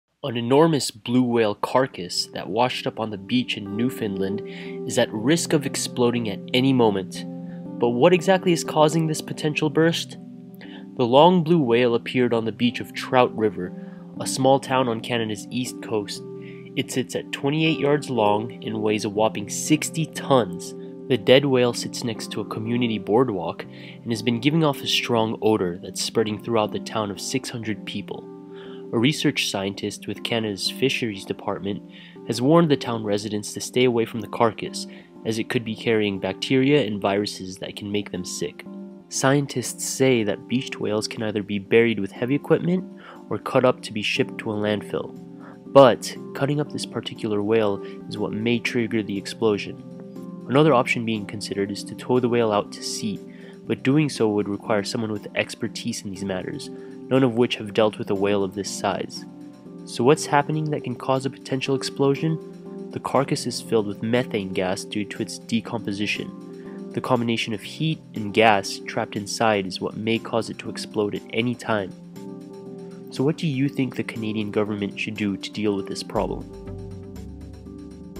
Giant 60-Ton Sea Creature
An enormous carcass of a giant sea creature washed up on the beach in Newfoundland is at risk of exploding at any moment. Identified as a 60-ton blue whale these are often mistake for huge sea monsters because of their size and decaying carcass. But what exactly is causing this potential burst?

The long blue whale appeared on the beach of Trout River, a small town on Canada's east coast. It sits at 28-yards long and weighs a whopping 60 tons. The dead whale sits next to a community boardwalk and has been giving off a strong odor that's spreading throughout the town of 600 people.

A research scientist with Canada's fisheries department has warned the town residents to stay away from the carcass as it could be carrying bacteria and viruses that can make them sick.

Scientists say that beached whales can either be buried with heavy equipment or cut up to be shipped to a landfill. But cutting up this particular whale is what may trigger the explosion. Another option being considered is to tow the whale out to sea, but doing so would require someone with expertise in these matters--none of which have dealt with a whale of this size.

So what's happening that can cause a potential explosion? The carcass is filled with methane gas due to its decomposition. The combination of heat and gas trapped inside is what may cause it to explode at any time.

So what do you think the Canadian government should do to deal with this problem?